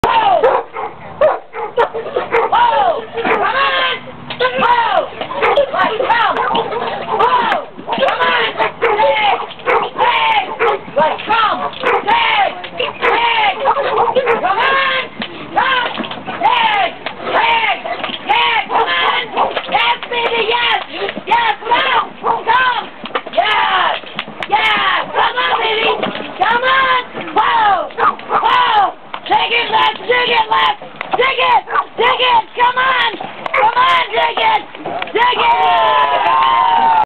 Less Paul Weightpulling
This is a picture of my mother's Great Dane, Less Paul weightpulling in Windber, PA.  This was his UKC record setting pull.  He successfully pulled 9,950 pounds in 35 seconds setting the UKC's weight pull record.